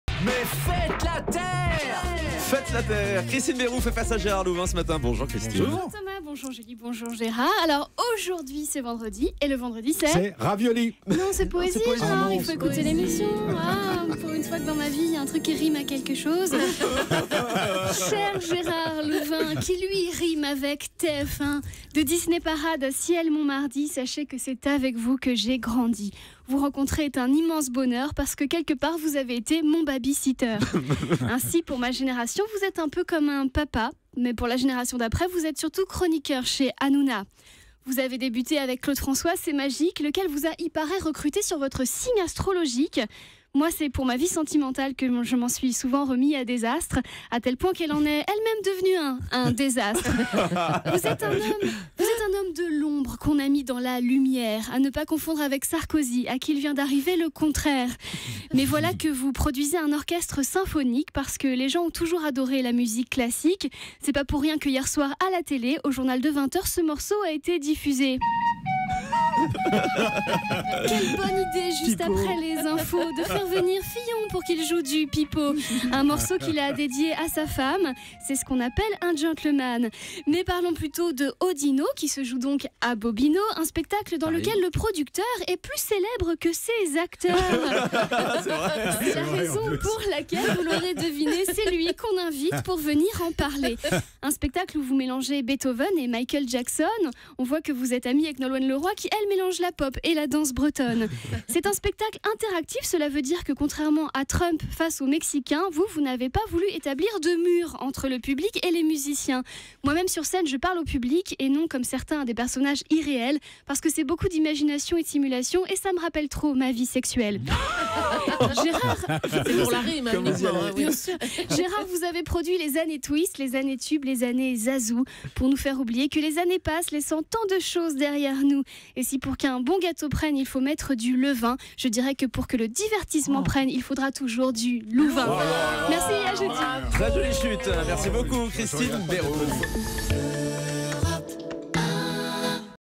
Pour que le divertissement prenne, il faut du Louvin
Christine Berrou est venu déclamer en vers et en rimes son adoration pour Gérard Louvin, le producteur qui a bercé sa jeunesse.